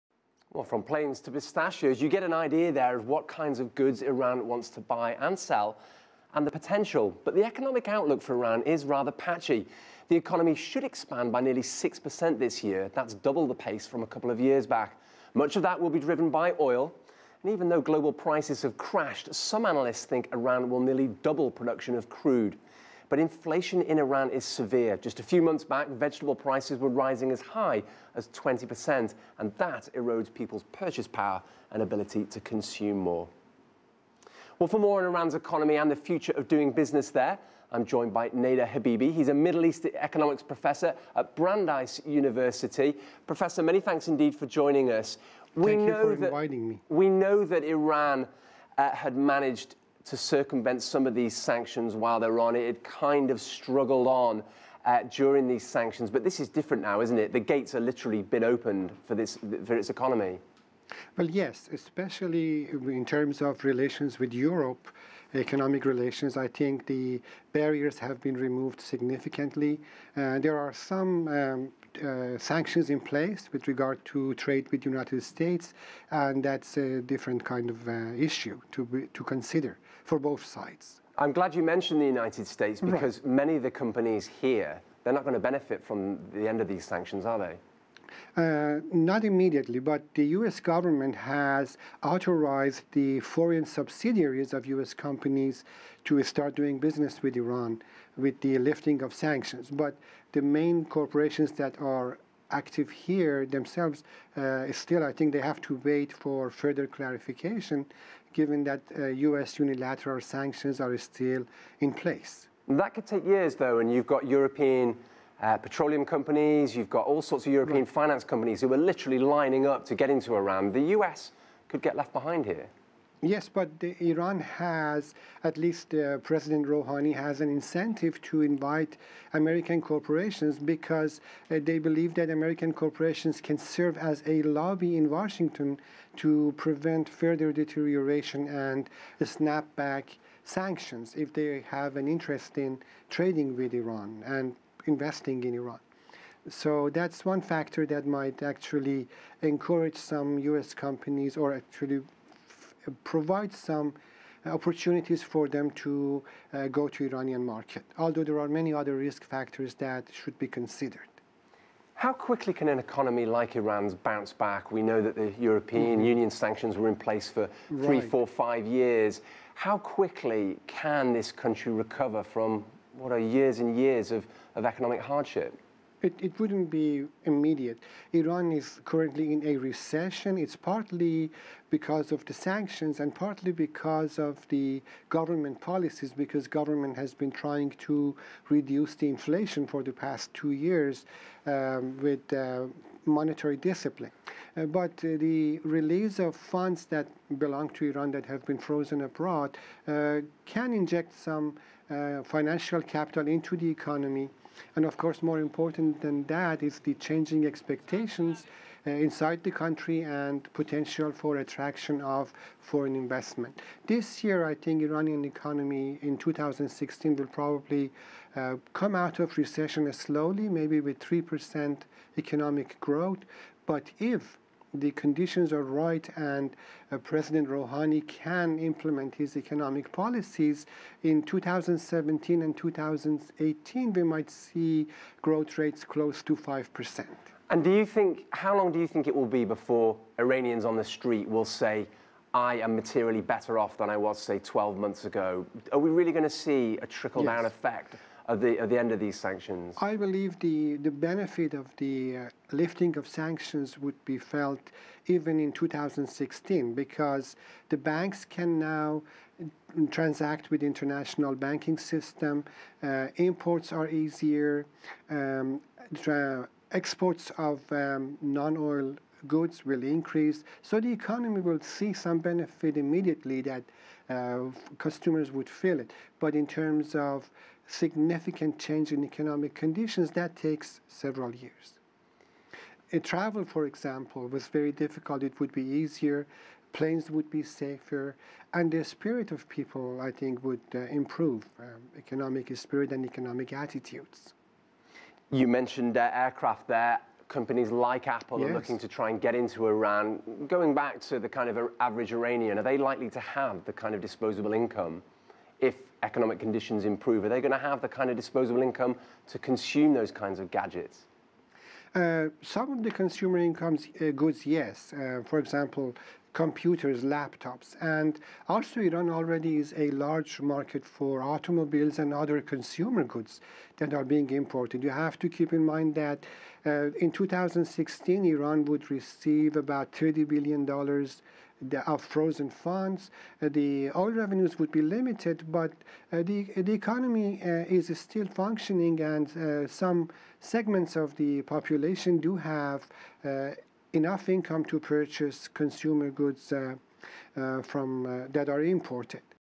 Middle East economist Nader Habibi on Iran's economic future
CCTV America's Owen Fairclough spoke with Nader Habibi who is a Middle East economics professor at Brandeis University.